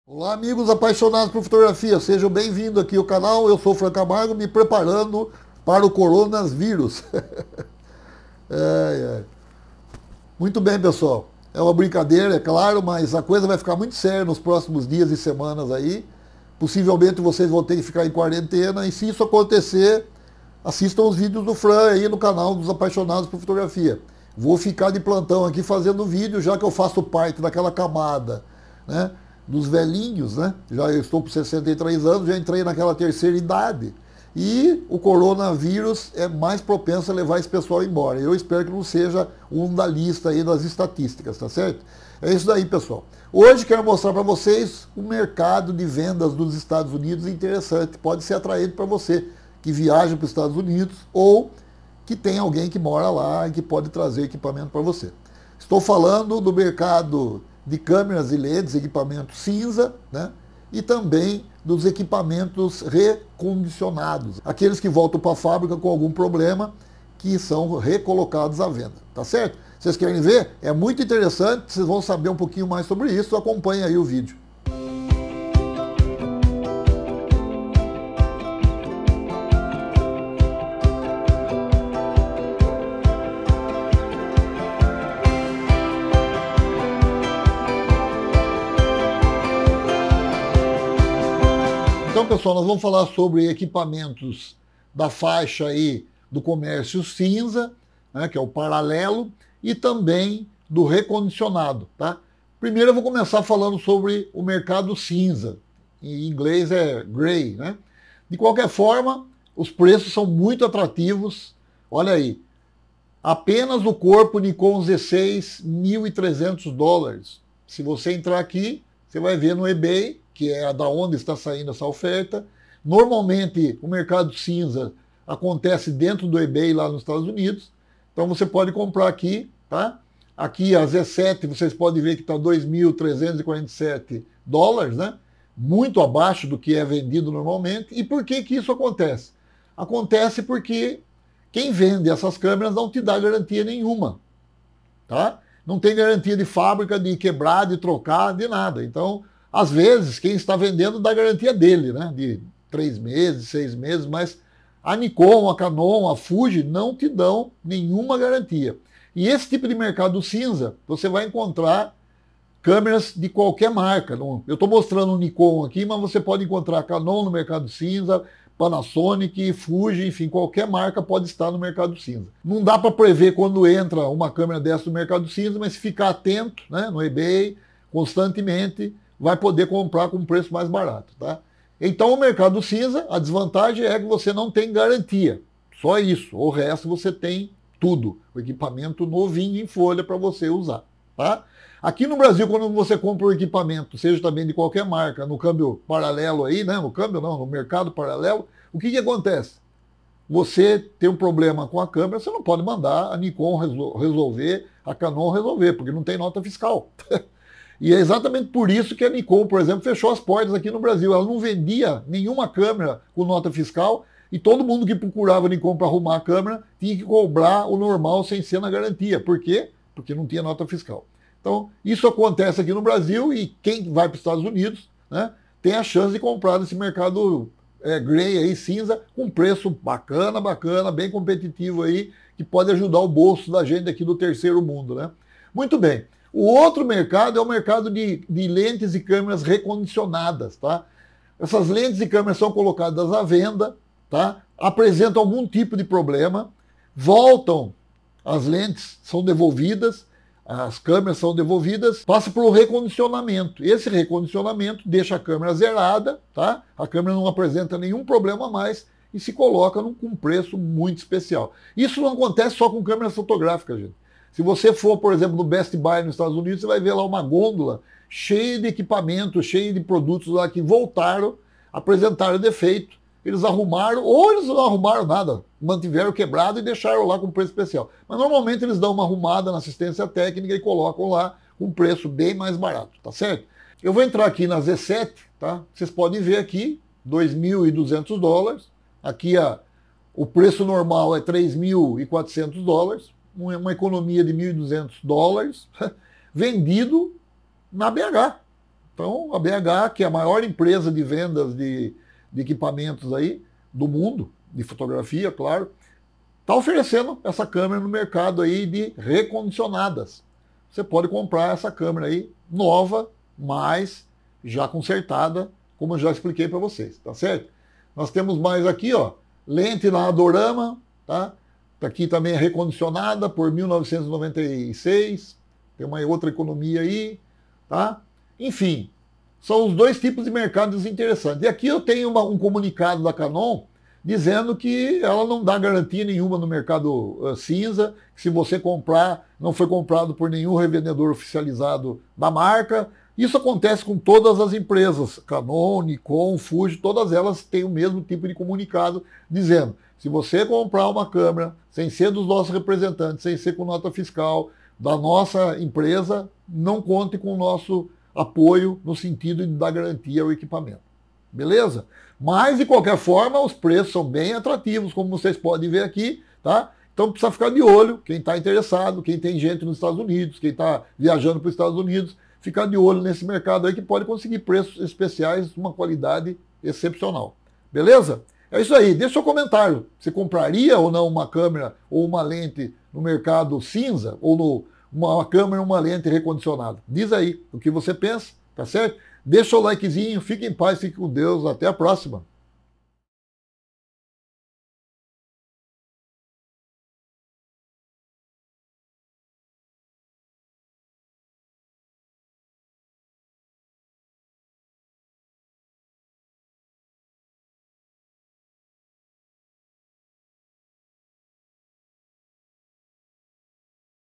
Você conhece os mercados Grey-Cinza e Refurbished - Recondicionados dos USA?
Os mercados Grey-Cinza e Refurbished - Recondicionados dos USA oferecem equipamentos novos com preços quase pela metade. Não é Fake News. Isso mesmo. Veja o vídeo e saiba mais...